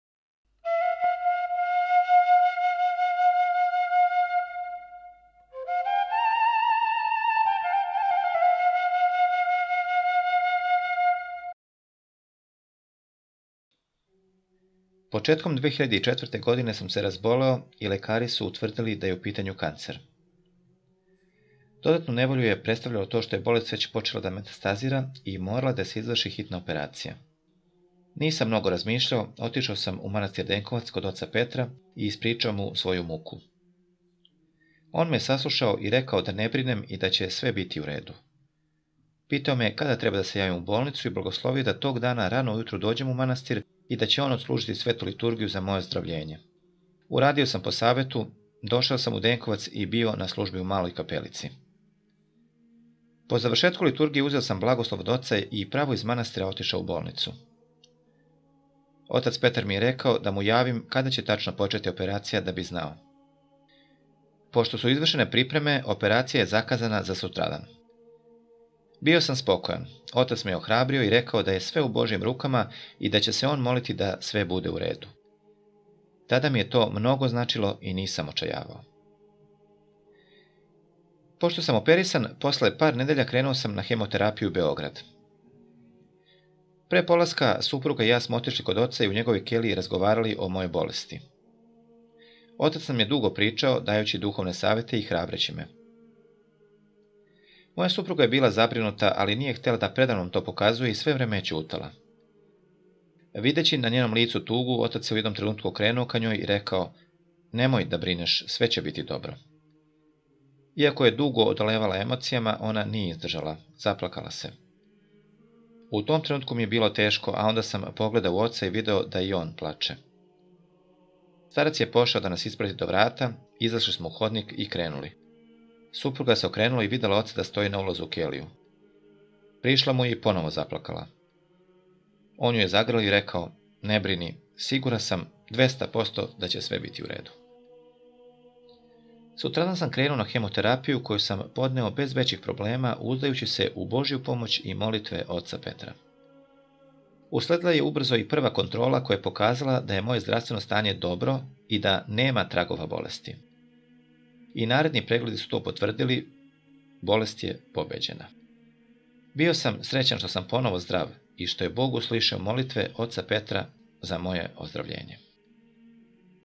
ИСЦЕЉЕЊЕ ОД КАНЦЕРА
Из књиге: ЖИВИ У ИСТИНИ - Сећања на схи-архимандрита Петра Денковачког, Крагујевац 2007.